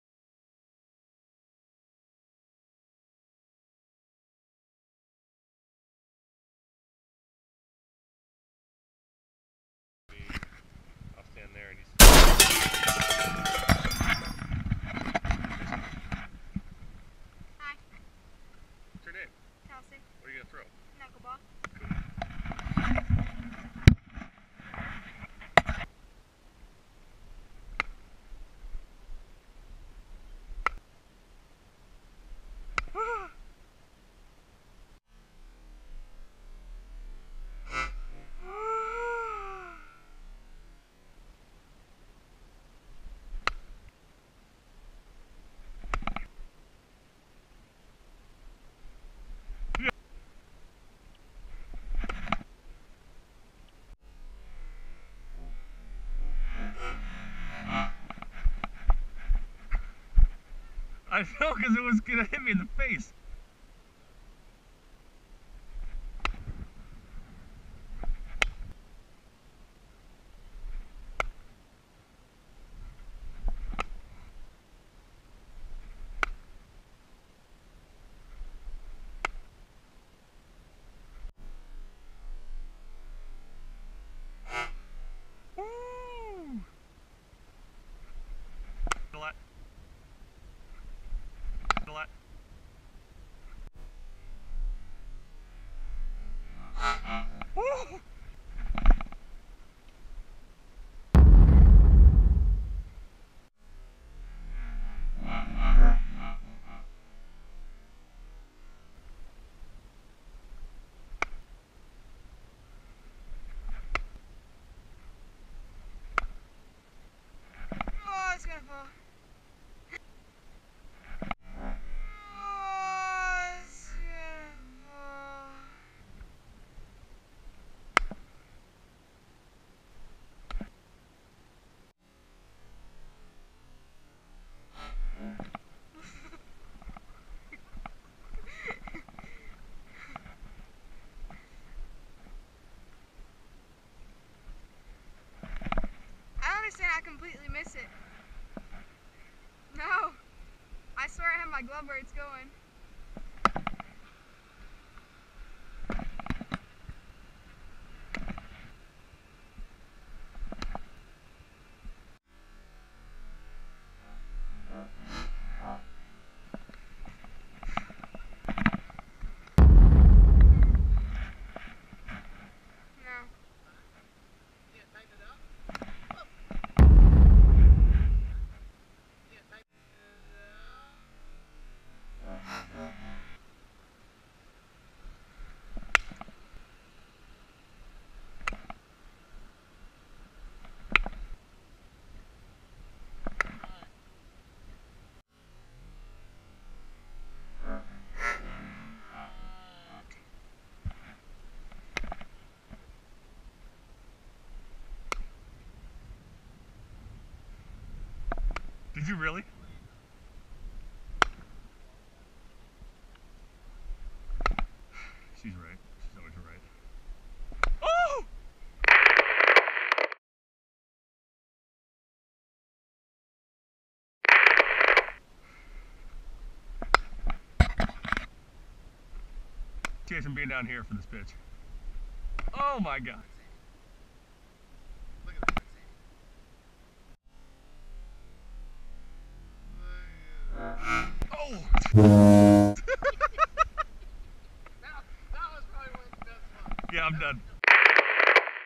Knuckleballing is NOT a crime - Frank Viola III vs Chelsea Baker
High School sensation Chelsea Baker throws her knuckleball with Toronto Blue Jays Knuckleball Pitcher Frank Viola III. Go Pro footage!